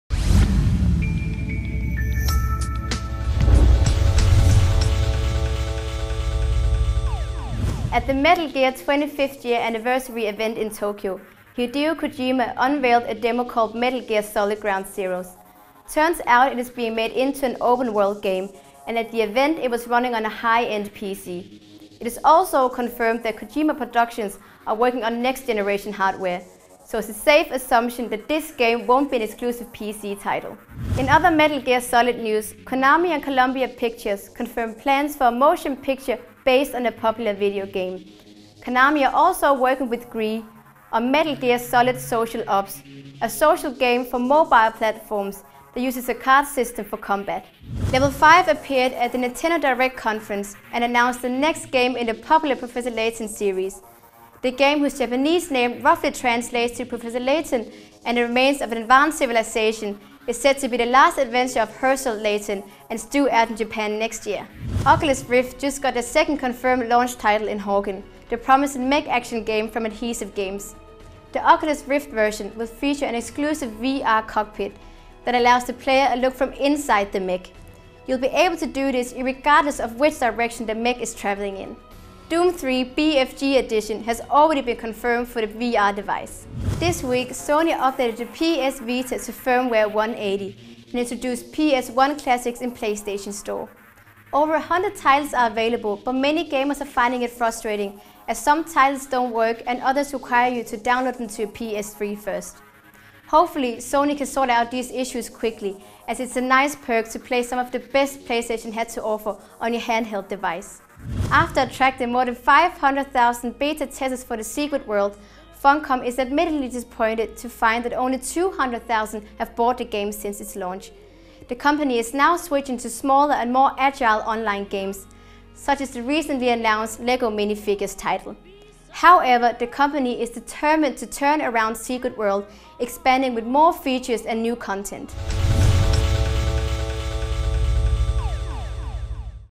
GRTV News - 31 August 2012
Emily brings us the latest news from the world of gaming including the future of Metal Gear Solid.